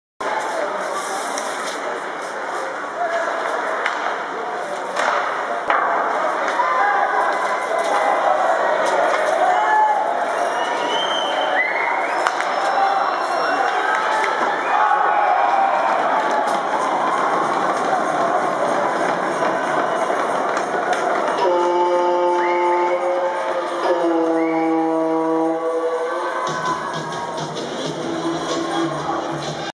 Teuvo Teravainen goal FIN 3:1 (FIN U17 - CZE U17 4:1)
European Youth Olympic Winter Festival  2011, Liberec, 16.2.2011
Finland U17 - Czech rep. U17 4:1
55:06 Goal Finland 3:1 20 TERÄVÄINEN Teuvo, as.: 12 MARKKULA Samu, as.: 27 TIKKINEN Niklas